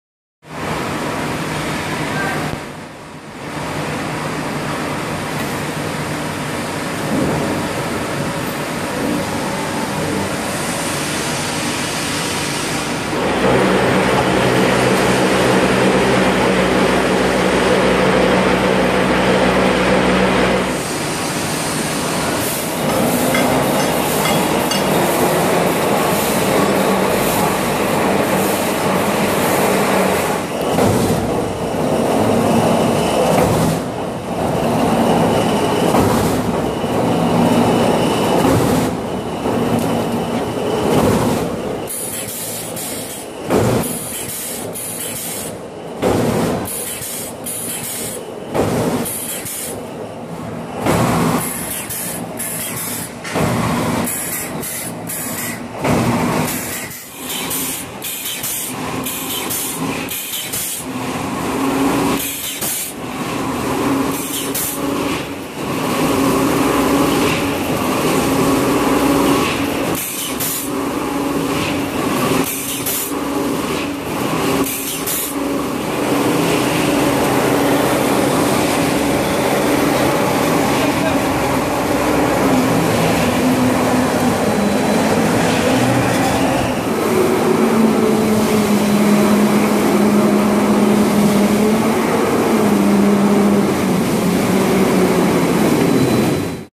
Log Debarker
This is a Rotary type De-Barker to remove the Bark of Log and to remove the top layer as Core Veneer. The De-Barker has Optical Centering and Charger fitted with it.